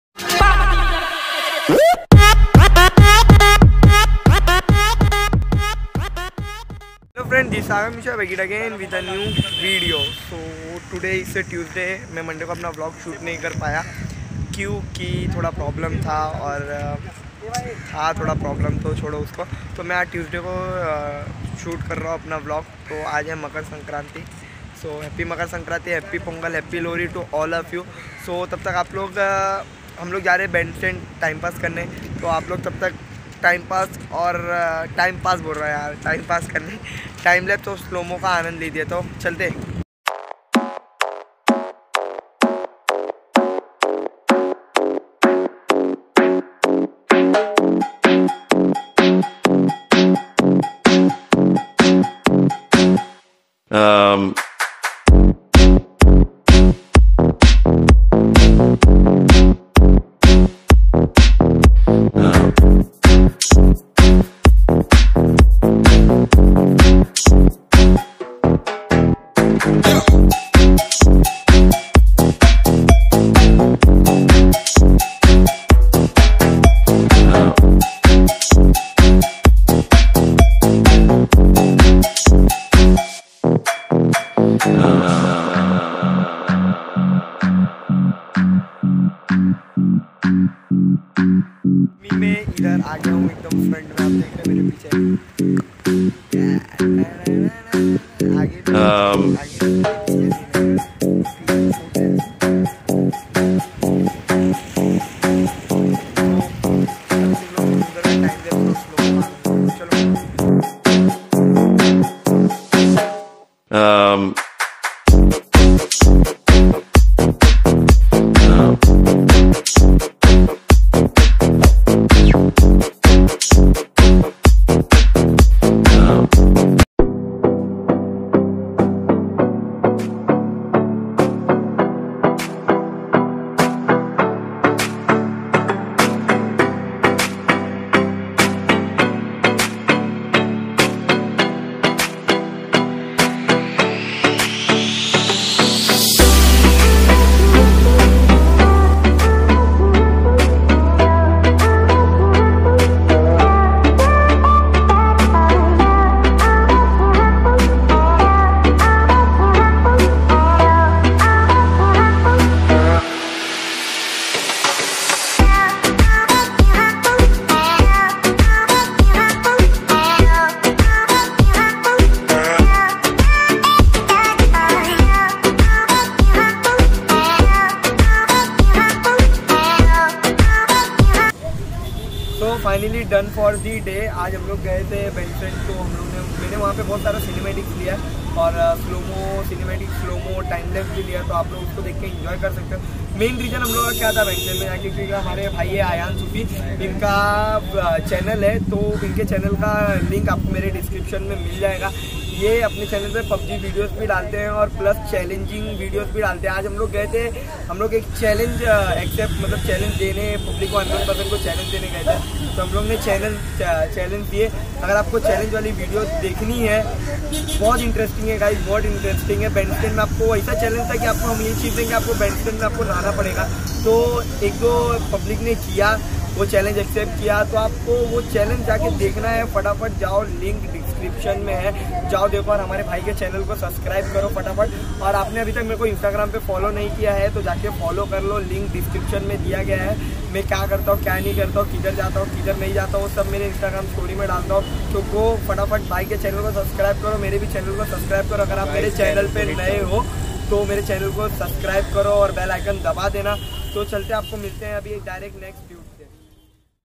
Giving challenge to girls | Bandstand | Sagar mishra
This video is all about life style vlog and challenge to unknows people...hope you all will enjoy this video..

This challenge is very crazy and we will bring many more challenge.